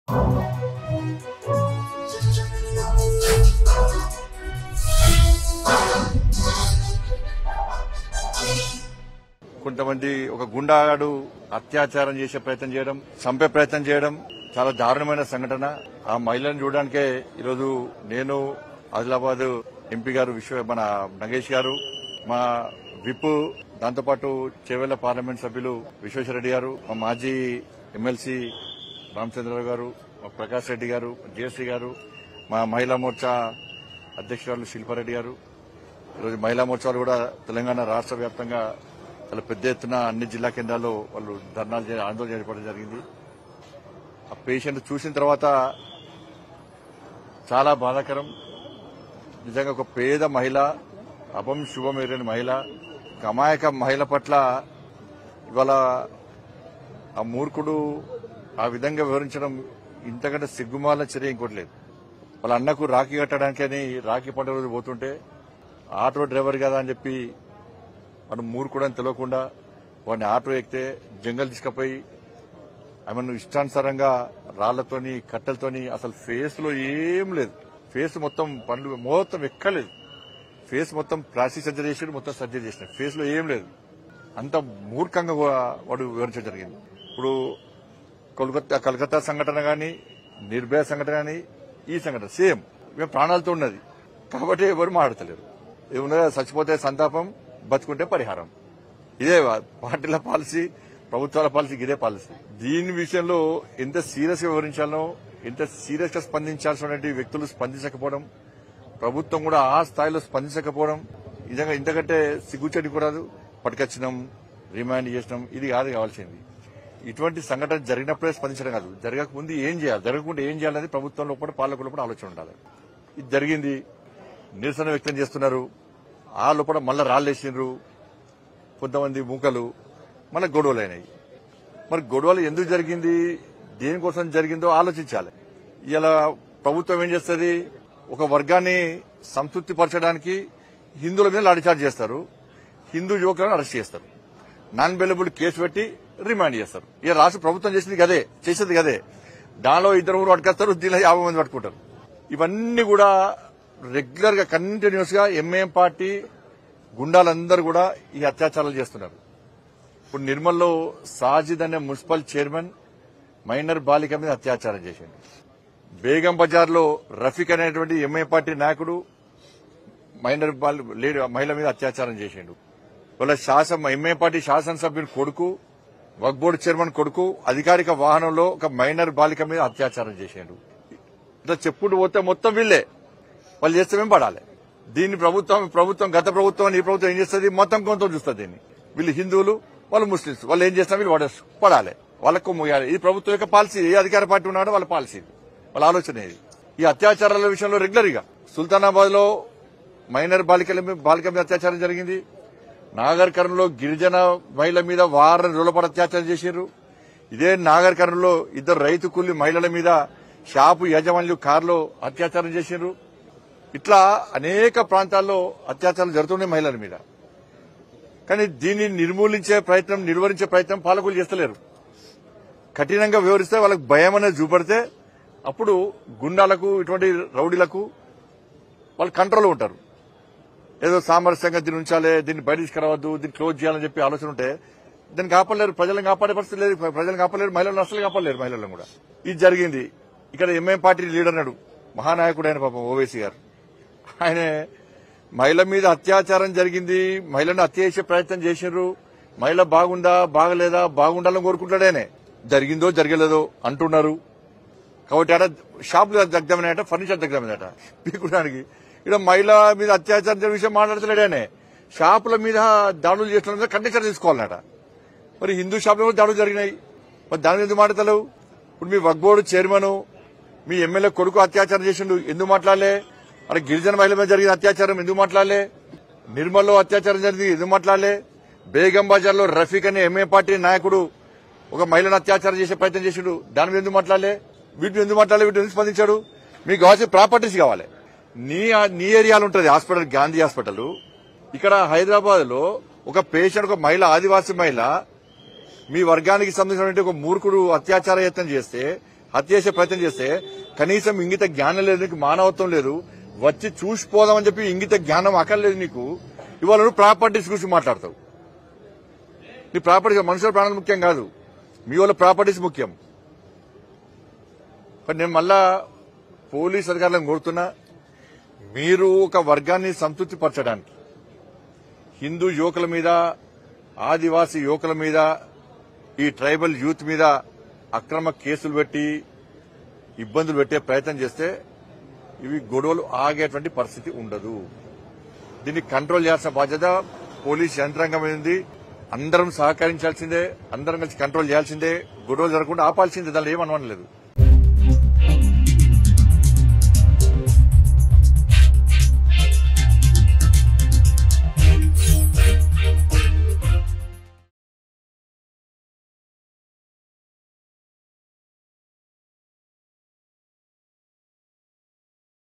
Union Minister Bandi Sanjay Sensational Comments On Jainoor Incident | BJP | Telangana Politics
Union Minister Bandi Sanjay Sensational Comments On Jainoor Incident. He demands the government take strong action against the culprits of the Jainoor incident.